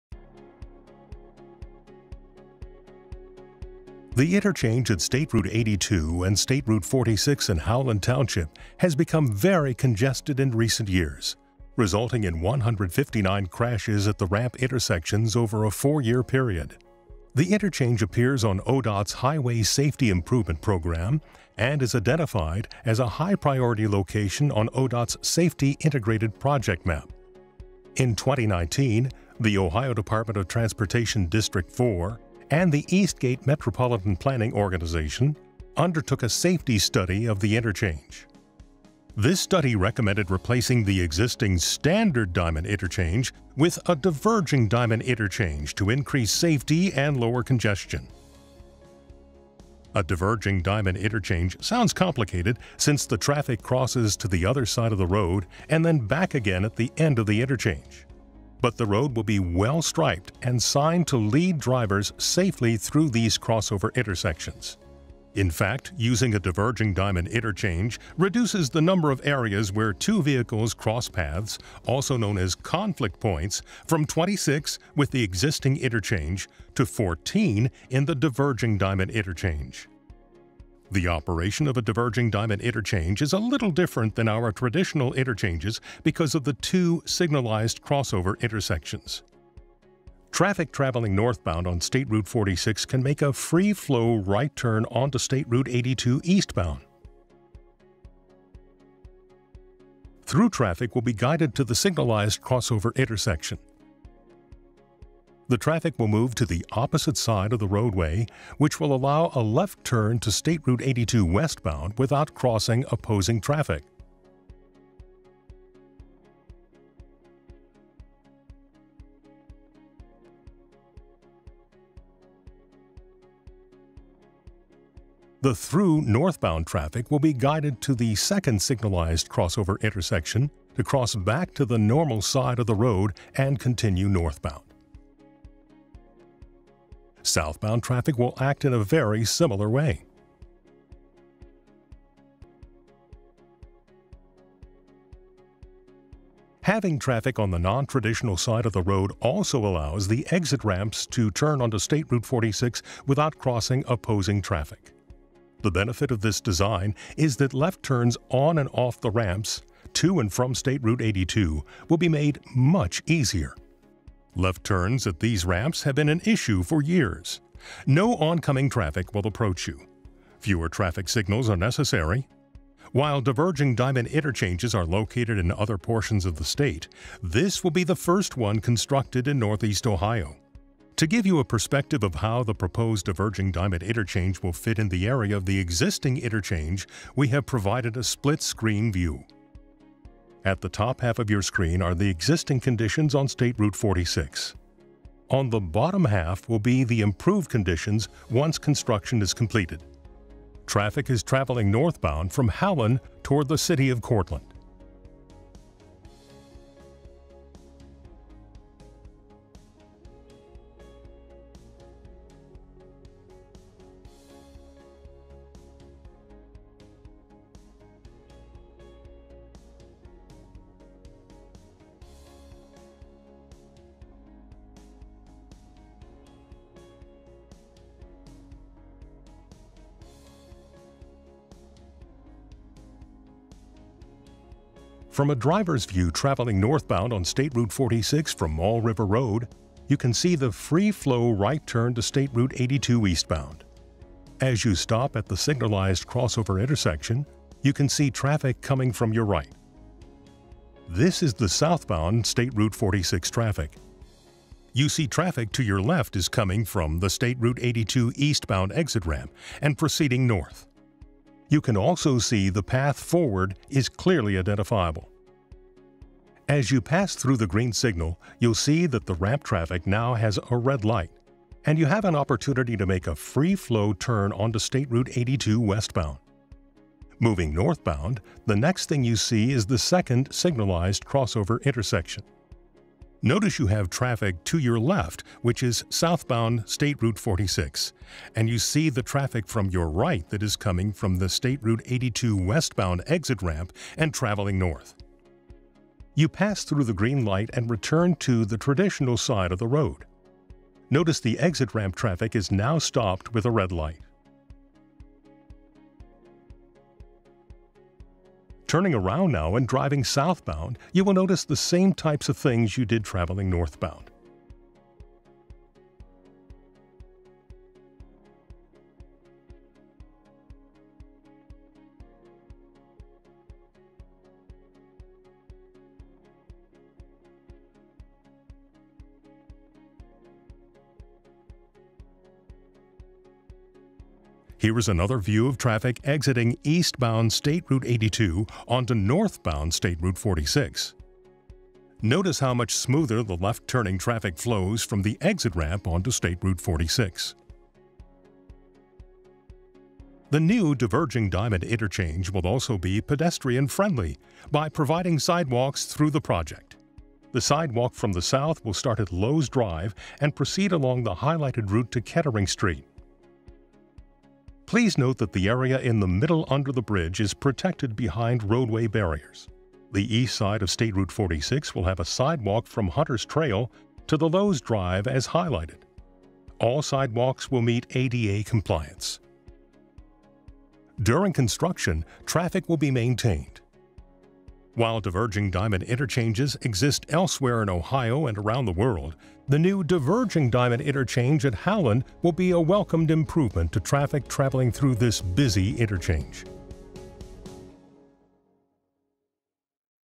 Diverging Diamond Interchange Trumbull County SR46/82
Challenge:
While Diverging Diamond Interchanges (DDI) reduce congestion and improve safety, their innovative design can be confusing when first encountered by the public.
ODOT wished to develop tools to inform the public on the benefits of the new design and how to properly navigate this new style in freeway interchange.

Solution:

CCI used the existing 3D OpenRoads design model to create a video simulation of the DDI.  Traffic simulation was derived form the OpenRoads microsimulation tools.
Video showed existing conditions and planned deign changes as well as a driver view immersive view of driving through the new interchange from various entry and exit points to the DDI.